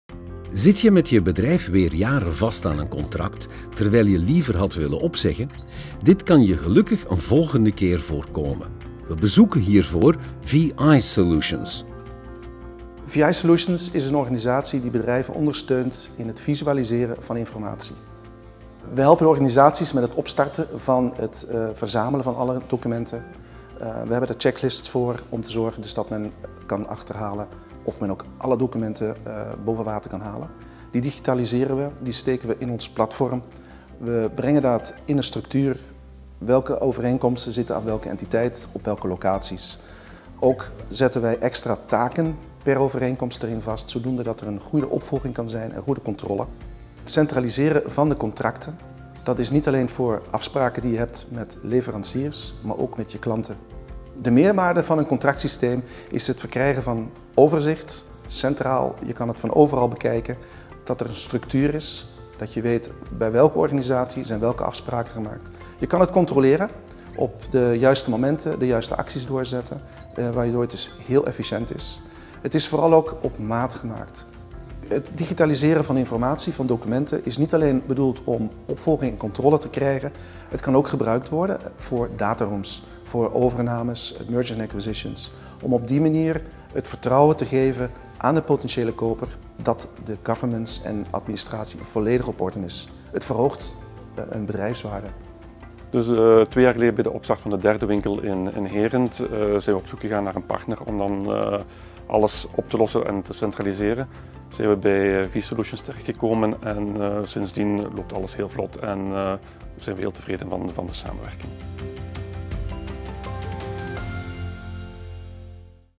VI Solutions
Zit je met je bedrijf weer jaren vast aan een contract, terwijl je liever had willen opzeggen? Dit kun je gelukkig een volgende keer kun je voorkomen, we bezoeken hiervoor VI Solutions!